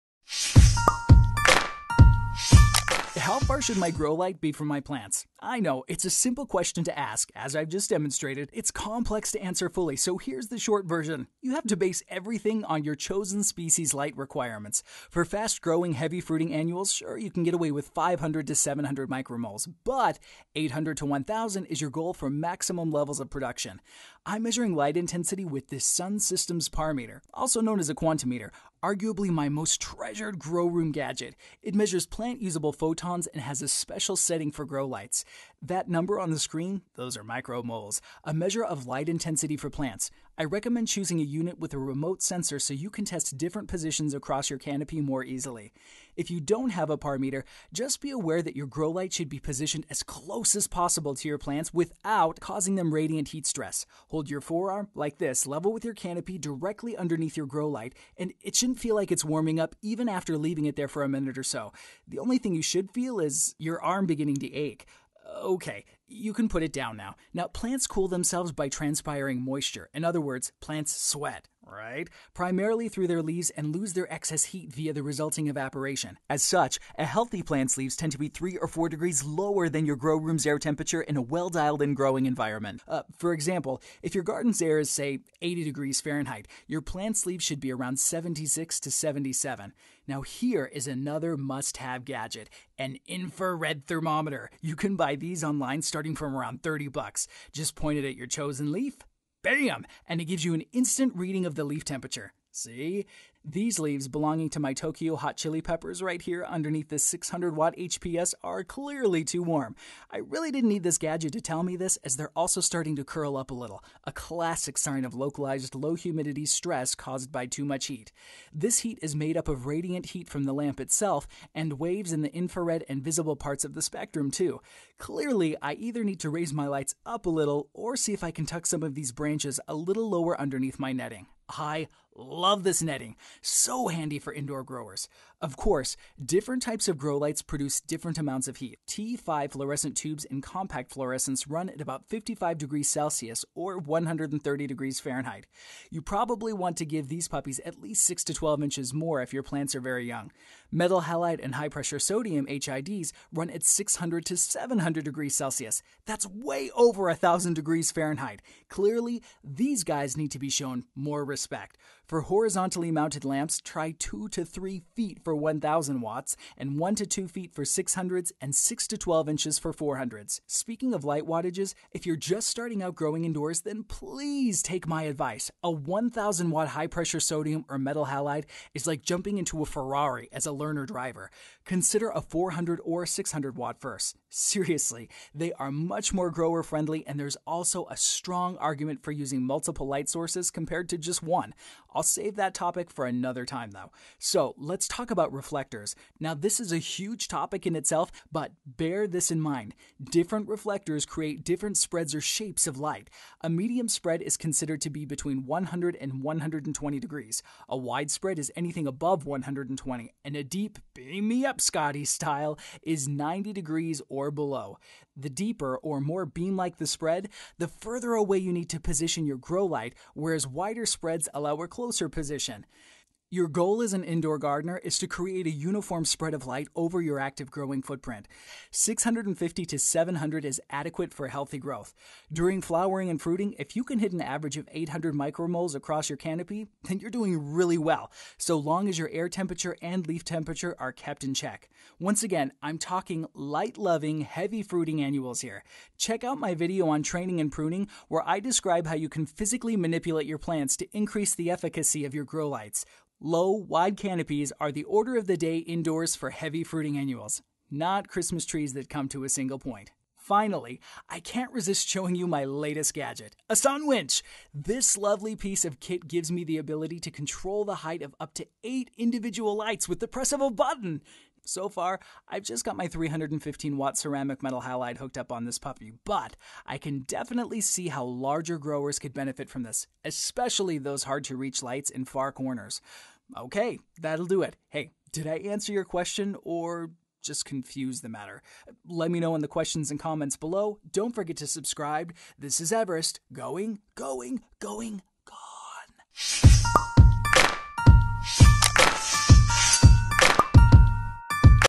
How Far Should My Growlight Be From My Plants?
Ever wondered what the optimal distance is between your grow light and your plant? We show you how to evaluate it for yourself, whatever plant species you are growing or type of grow light you are using by using an infrared thermometer and PAR light meter.